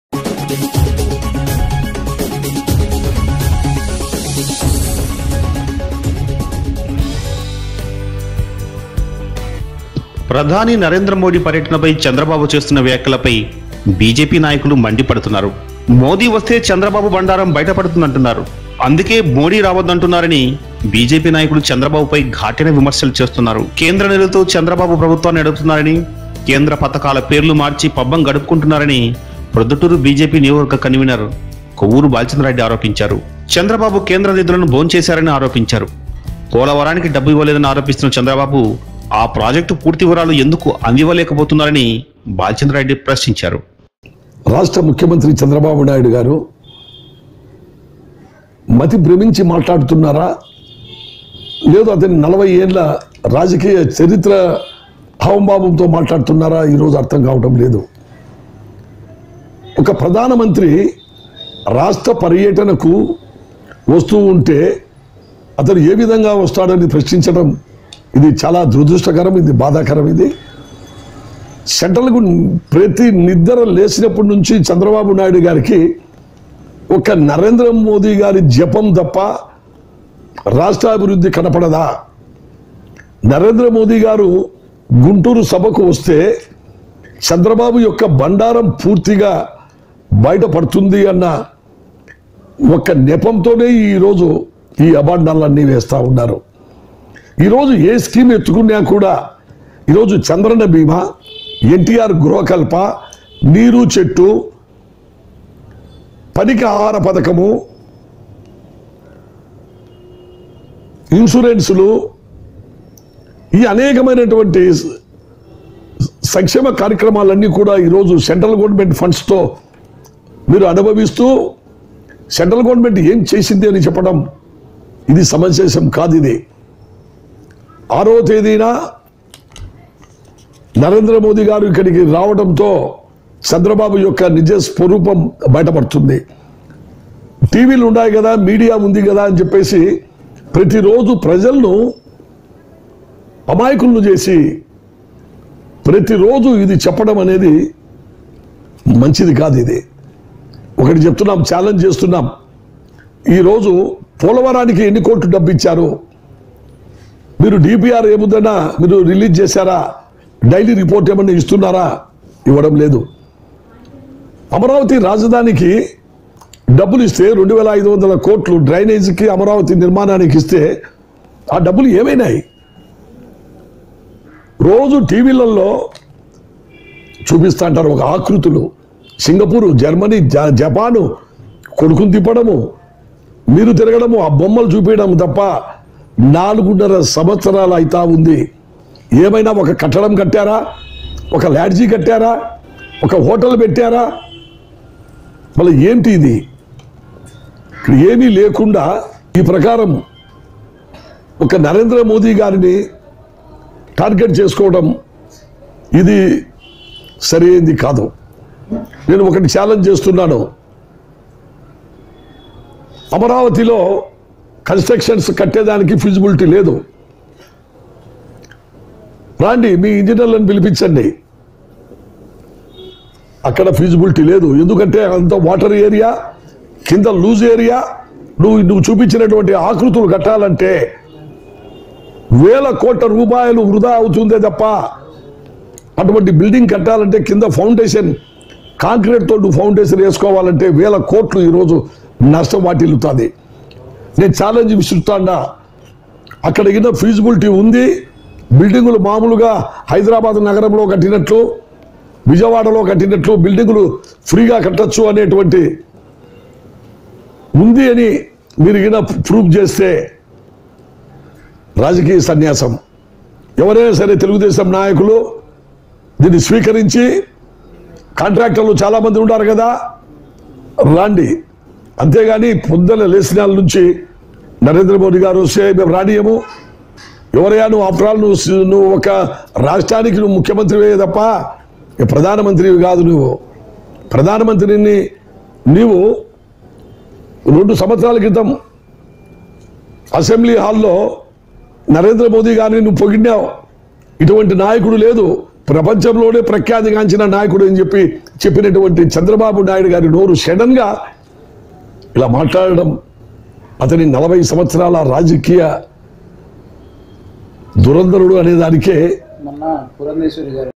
చంద్ర‌బాబుపై బిజెపి నేత‌ల ఫైర్‌|| BJP LEADERS FIRE ON CHANDRABAU
ప్ర‌ధాని న‌రేంద్ర మోడీ ప‌ర్య‌ట‌న‌పై చంద్ర‌బాబు చేస్తున్న వ్యాఖ్య‌ల‌పై బిజెపి నాయ‌కులు మండి ప‌డుతున్నారు. మోదీ వ‌స్తే చంద్ర‌బాబు బండారం బ‌య‌ట‌పడుతుందంటున్నారు. అందుకే మోదీ రావొద్ద‌ని అంటున్నార‌ని బిజెపి నాయకులు చంద్ర‌బాబుల‌పై ఘాటైన విమ‌ర్శులు చేస్తున్నారు. కేంద్ర నిధుల‌తోనే చంద్ర‌బాబు ప్ర‌భుత్వాన్ని న‌డుపుతున్నార‌ని, కేంద్ర ప‌థ‌కాల పేర్లు మార్చి పబ్బం గ‌డుపుకుంటున్నార‌ని ప్రొద్దుటూరు బిజెపి నియోజ‌వ‌ర్గ  క‌న్వీన‌ర్ కొవ్వూరు బాల‌చంద్రారెడ్డి ఆరోపించారు. చంద్ర‌బాబు కేంద్ర నిధుల‌ను భోంచేశార‌ని ఆరోపించారు. పోల‌వ‌రానికి డ‌బ్బు ఇవ్వ‌లేద‌ని ఆరోపిస్తున్న చంద్ర‌బాబు ఆ ప్రాజెక్టు పూర్తి వివ‌రాలు ఎందుకు అందివ్వ‌లేక పోతున్నార‌ని బాల‌చంద్రారెడ్డి ప్ర‌శ్నించారు.